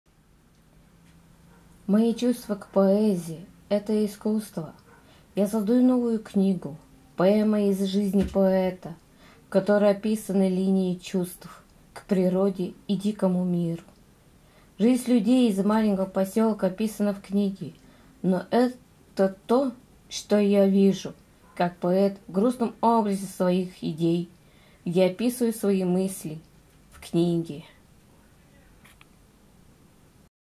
Мысли в слух
I am describing a love for nature, animals and the world that surrounds us. Much has been said about the world of wild forests, where wild animals live freely. I have talked many times about conservation of nature and wild animals that brutally kill and destroy trees. The natural world is beautiful when we admire the beauty, but there is no need to disturb the forest animals. Let nature delight people with its beauty, and let forest animals be happy and walk freely in the wild forest.